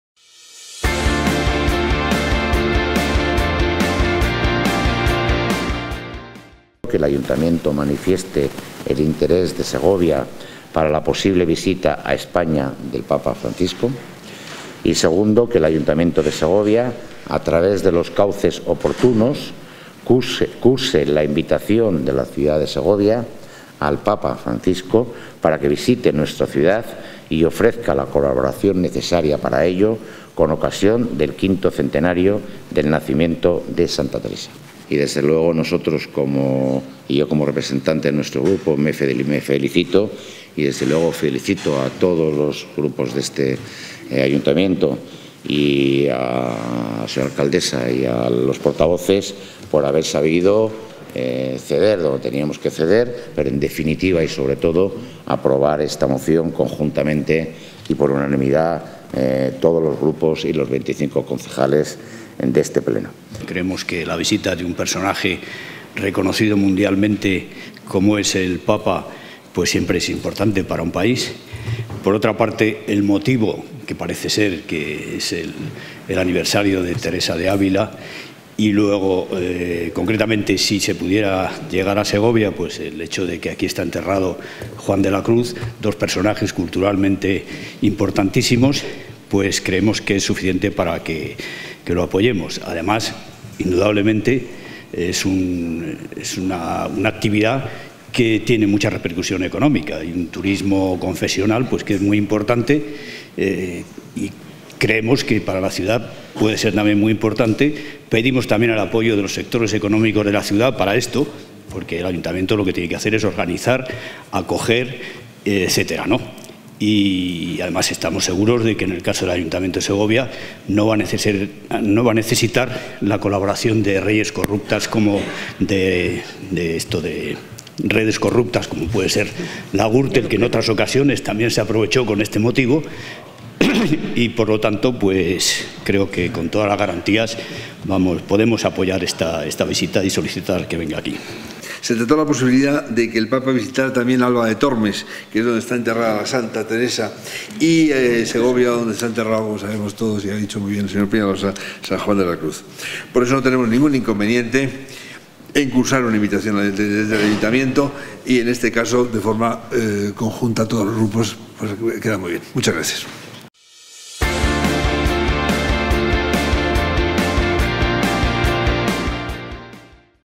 El Ayuntamiento aprueba por unanimidad invitar oficialmente al Papa Francisco a visitar Segovia
El Ayuntamiento, reunido en el Pleno ordinario de mayo este viernes, ha aprobado por unanimidad invitar oficialmente al Papa Francisco a visitar Segovia, aprovechando su  posible visita a España con motivo del V Centenario del nacimiento de Santa Teresa.